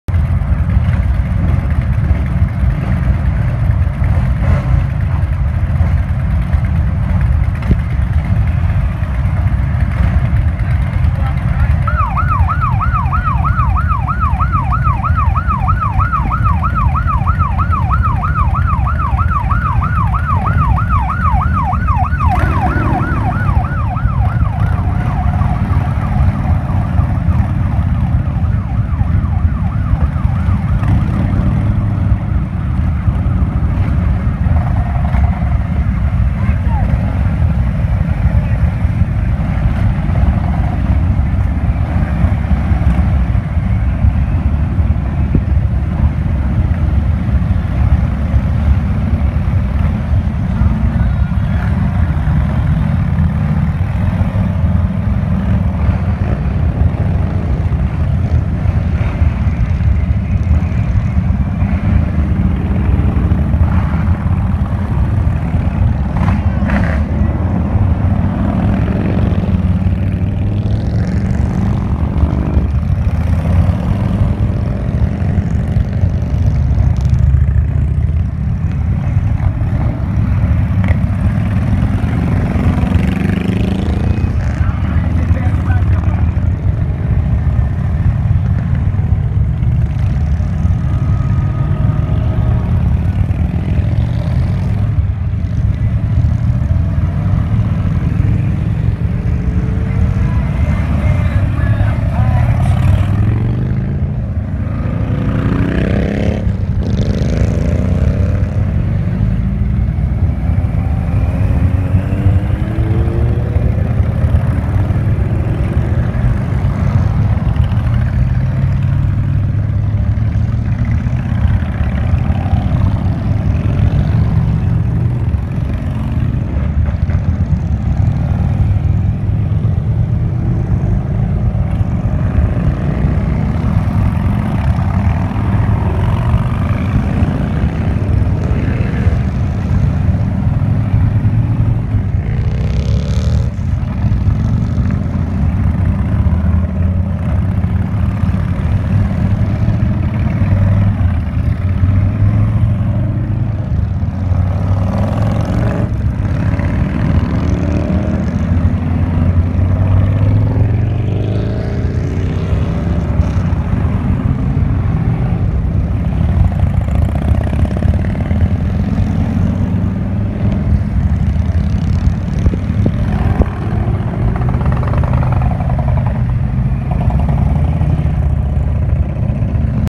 Fallen Brothers and Sisters Ride September 25, 2016
Fallen Brothers and Sisters Ride 9-25-16 Departing from Southern Lanes in Alexandria, Ky